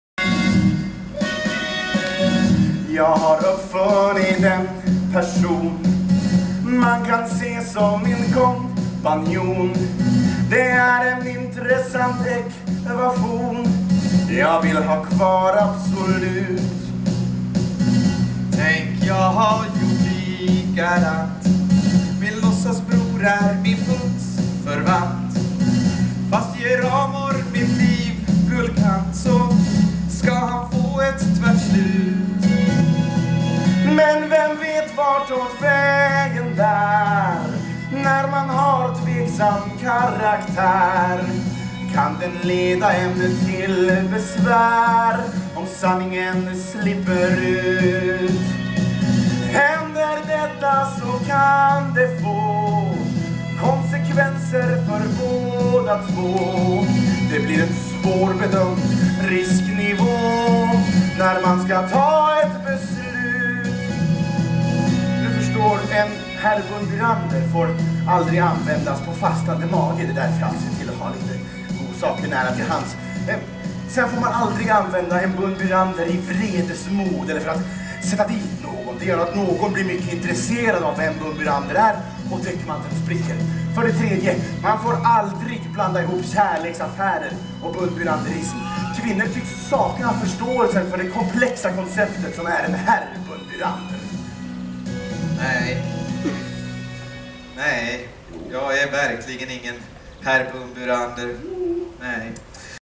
"I Ärlighetens Namn" - SommarTeatern 2012
Inspelat på Parksnäckan i Uppsala 12 juli 2012
Medverkande: Johan Norell och Simon Bergström